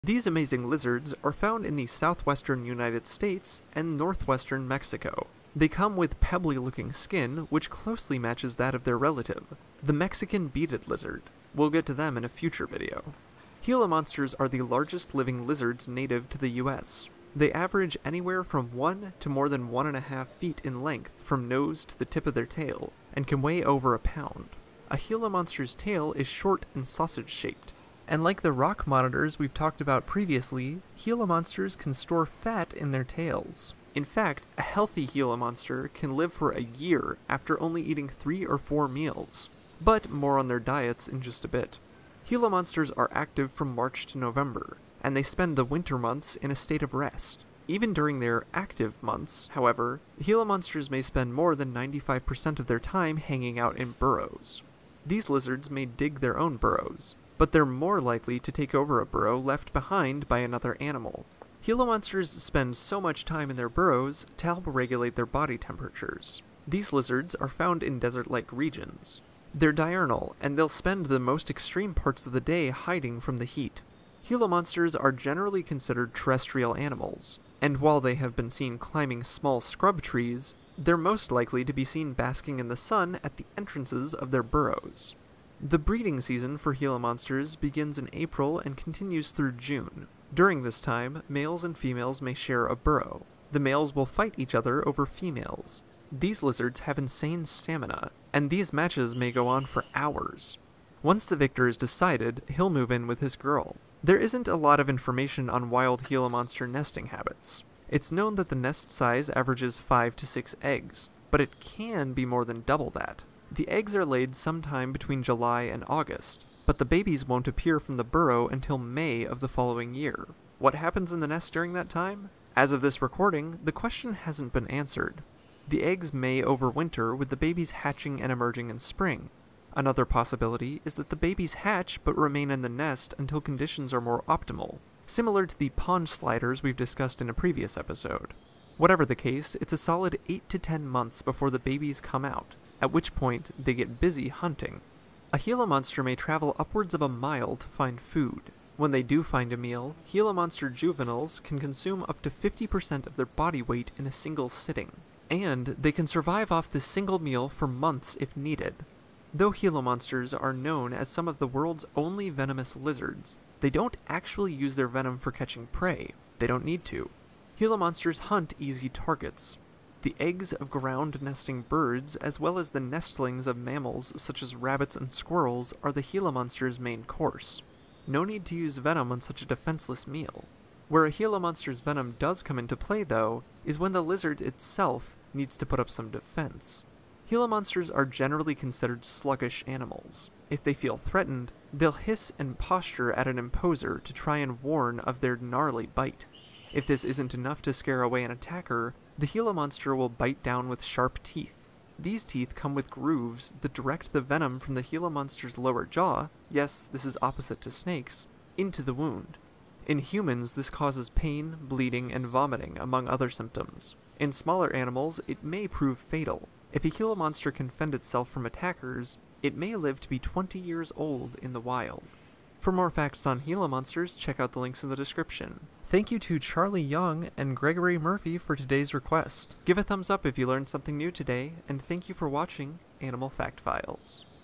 Gila Monster facts: lizards with venom | Animal Fact Files
Gila monsters are the largest native lizards living in the United States. They're also known as some of the only lizards in the world who are venomous, but they don't use this venom for catching prey. Gila monsters are generally considered sluggish animals and may spend months without eating. This is because they're really good at storing energy! Gila monsters live in burrows going so far as to lay their eggs inside of them.

Scientific Name: Heloderma suspectum
Range: southwestern US and northwestern Mexico
Size: 16.7 inches (425mm); 1.3 pounds (600g)
Diet: vertebrate eggs and nestlings
Lifespan: 20+ years